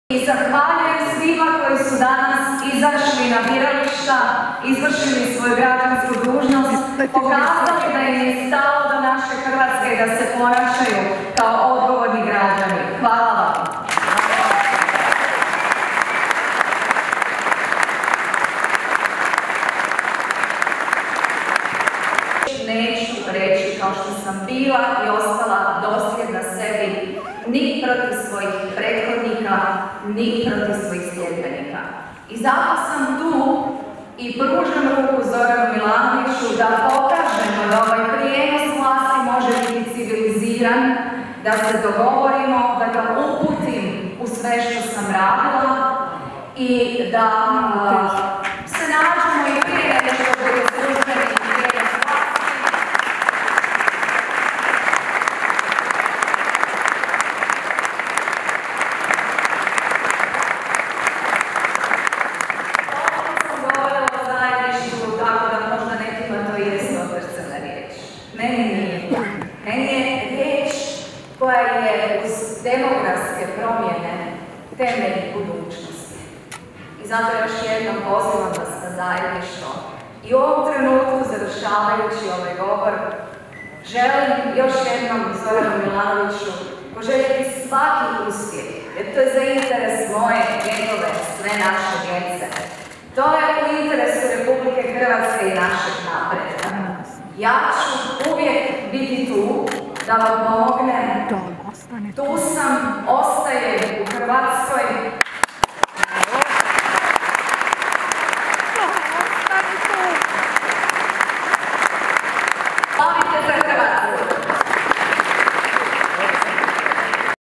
Grabar-Kitarović čestitala Milanoviću i zaželjela mu uspjeh u obnašanju predsjedničke dužnosti
Zagreb, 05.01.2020. - Kolinda Grabar-Kitarović obratila se okupljenima u svom stožeru. Predsjednička kandidatkinja HDZ-a i aktualna predsjednica čestitala je Zoranu Milanoviću na pobjedi na predsjedničkim izborima i zaželjela mu uspjeh i svako dobro u obnašanju dužnosti. video HINA/ Lana SLIVAR DOMINIĆ/ lsd